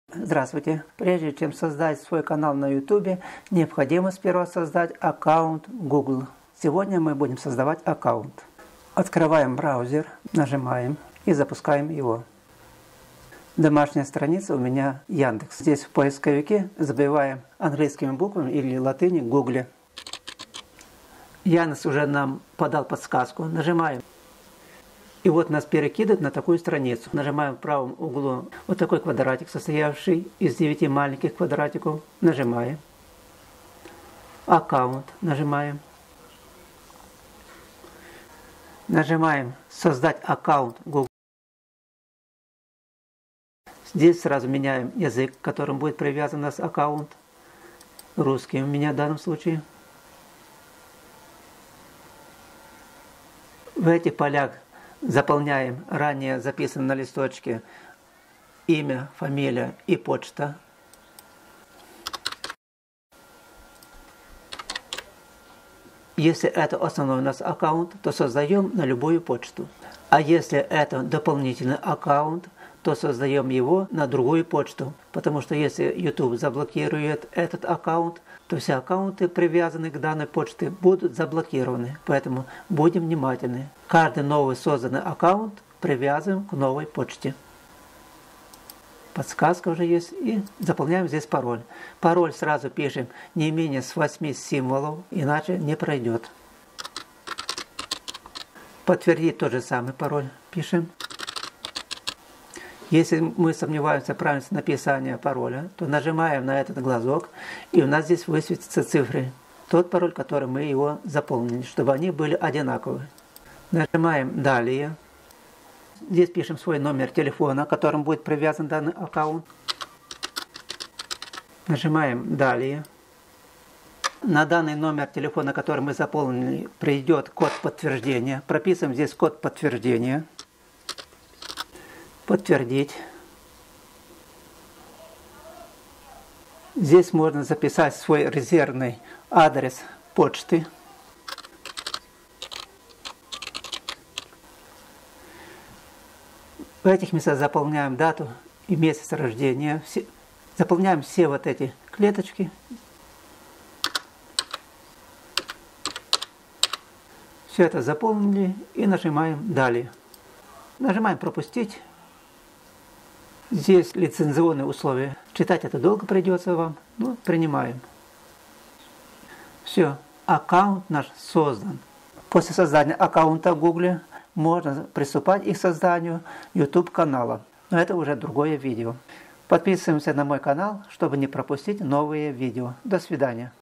Как создать новый аккаунт гугл .Правильный подход к созданию аккаунта в ютубе
Как правильно создать аккаунт в google.В этом видео рассмотрим как создать новый аккаунт гугл на кп. Обязательная регистрация в гугле,создать аккаунт без этого нельзя.Все тонкости как это сделать показано здесь. Некоторых начинающих блогеров интересует можно ли создать аккаунт гугл на кп,если уже один имеется.Многие спрашивают как создать аккаунт в ютубе.Это одно и то же.Да,можно.Только создаем новый аккаунт гугл привязываем его к новой почте,потому,что если ютуб заблокирует один аккаунт,то все аккаунты привязаны к этой почте будут заблокированы.Как создать аккаунт гул есть много видео выложенных в интернет.Но ютуб с каждым годом изменяет сам подход к тем или другим действиям к которым мы привыкли.Идёт сплошная путаница по созданию нового аккаунта гугл,и для начинающих -это задает некоторые трудности.Как создать новый аккаунт гугл года нам придётся,то за благовременно всё подготавливаем,и только тогда создаем его. Старый аккаунт гугл,который нам не нужен или заблокирован-удаляем.Как удалить старый аккаунт гугл на моем канале на эту тему есть видео. Теперь вам известно как создать новый аккаунт гул на компьютере.Создаём правильно аккаунт в google.Удачи!